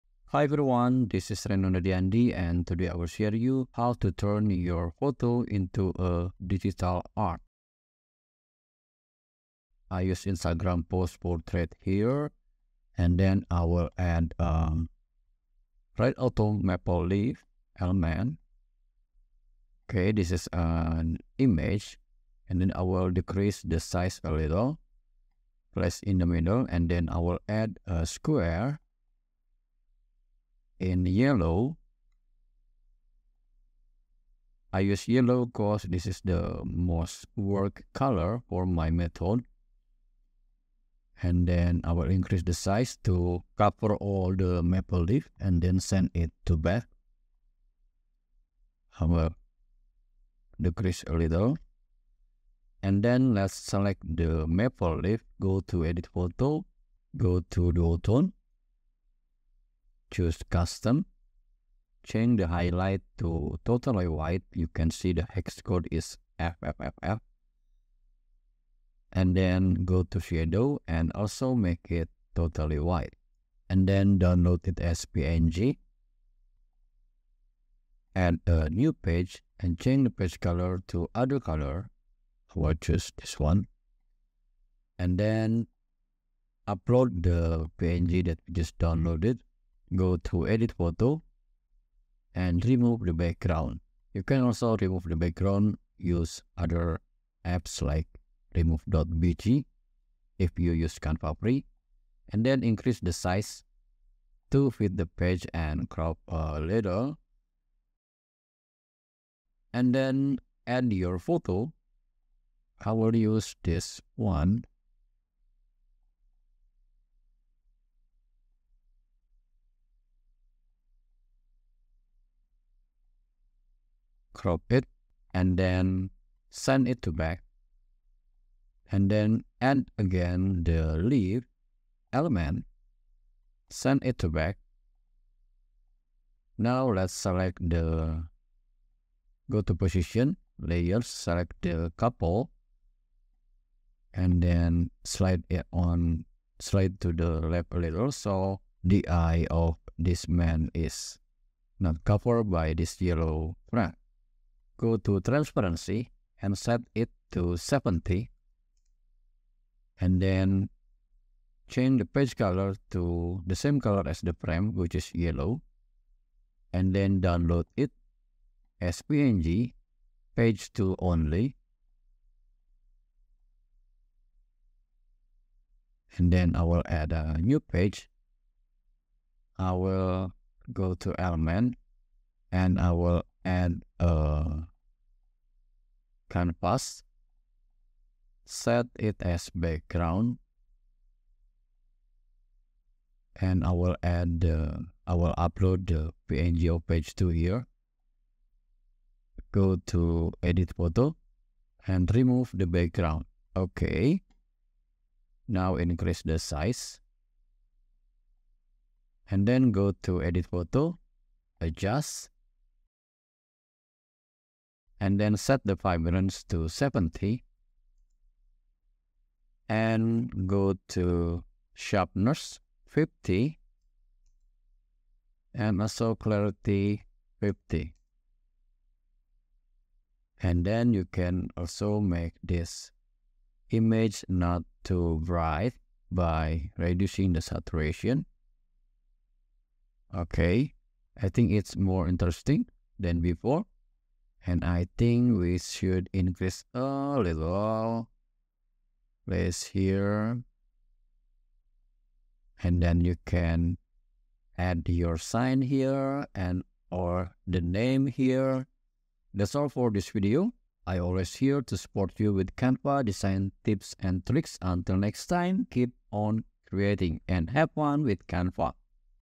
Canva Tutorial Digital Art | Insert Photo to Maple Leaf
In This Canva tutorial video you will learn how to create digital art in 3 steps. The topic is to fill leaf with photo, so it is a photo manipulation tutorial. And this is an update for this video

I hope this Canva design idea gives you more insight to add new skill so you can also add more to your graphic design portofolio. I made this step by step tutorial as Canva tutorial for beginners for all Canva lovers in the Australia, USA, Canada, United Kingdom and all around the globe.

The video chapters:
1. Create Custom frame from the maple leaf image
2. Fill the leaf frame with photos
3. Finishing the digital art by adjust the result photo and adding canvas as background

That's all for this video! Remember,  I always here to support you with Canva design tips and tricks. Until next time, keep on creating!